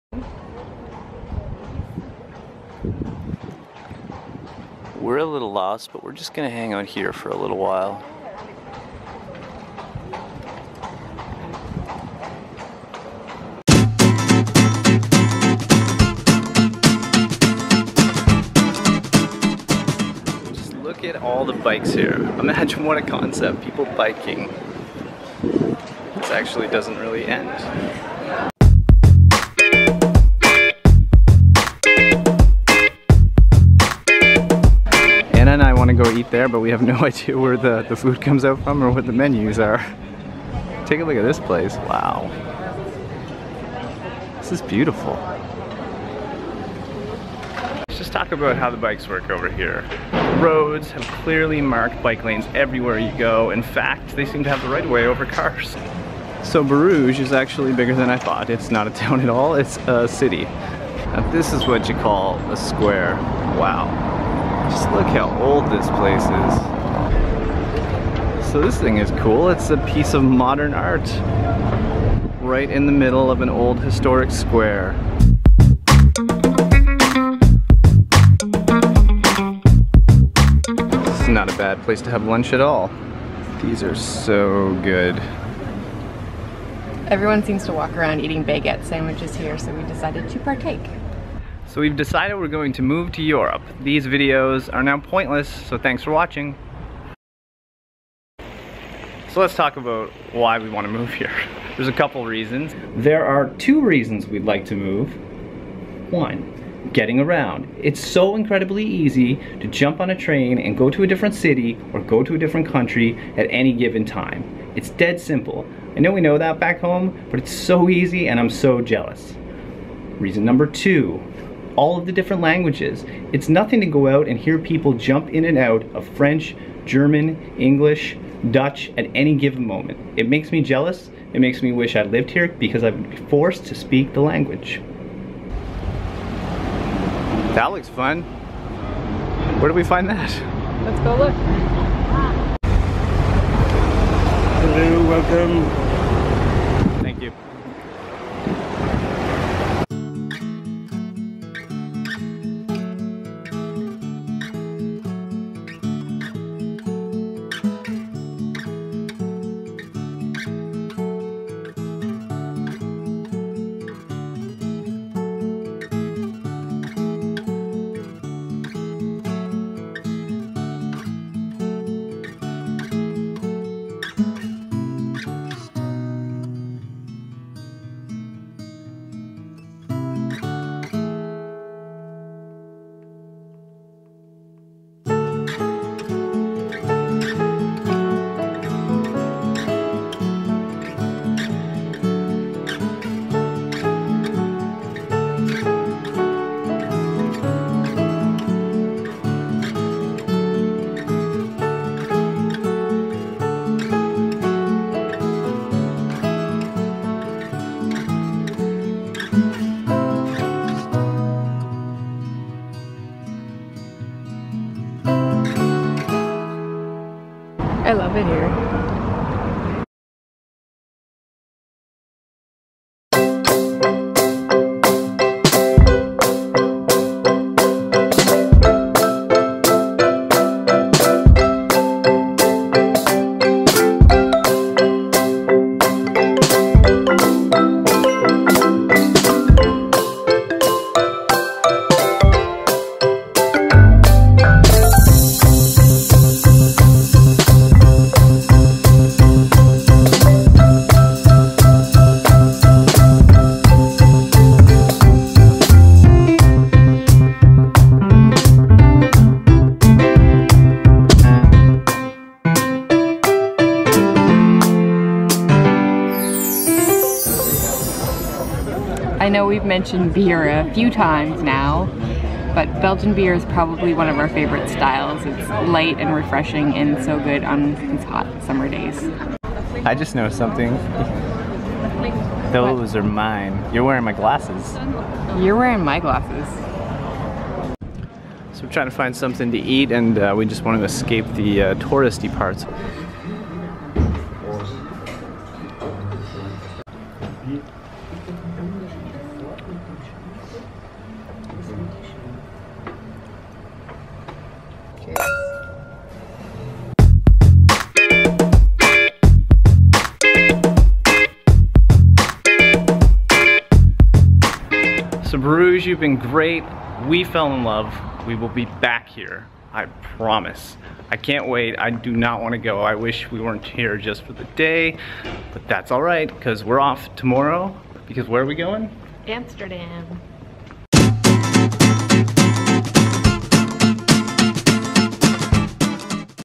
BRUGES. Amazing CANAL BOAT TOUR in this fairy tale city.
Bruges is easily one of our favourite cities. Come explore it's charm, take a canal boat tour and find some great places to eat.